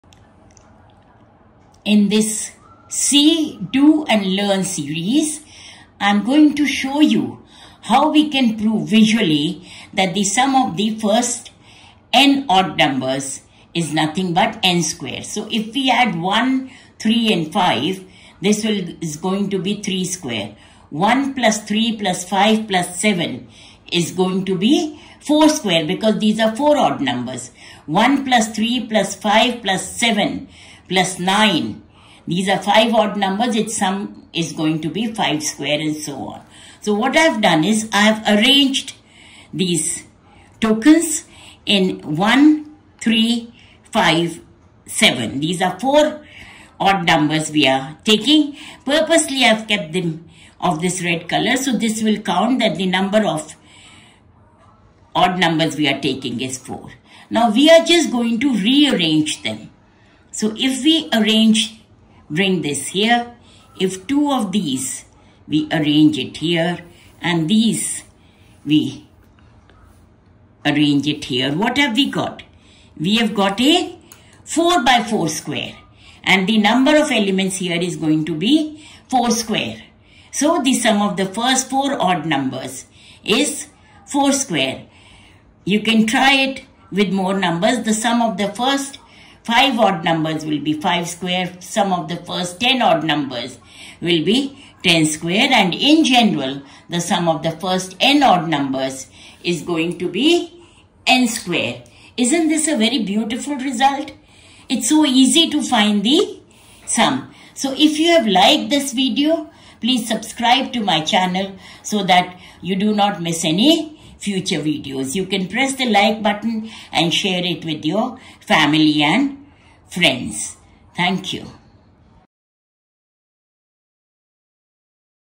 Why Odd Numbers Add Up to Perfect Squares! 🤯 | See Do & Learn It
1 + 3 + 5 + 7 + ...
Ever noticed something magical?
🎯 In this short visual explainer, we reveal the secret behind the sum of the first n odd numbers — and it’s beautifully simple:
1 + 3 + 5 + ... + (2n–1) = n²!

✨ With colorful visuals and a geometric twist, this isn’t just math — it’s math you can see, do, and never forget.

👀 Perfect for:

Curious students (Class 5 and above)

Math teachers bringing concepts to life

Parents, puzzle-lovers, and visual learners

Anyone who loves spotting patterns and symmetry!

🎥 Short, sharp, and stunning — just how math should be.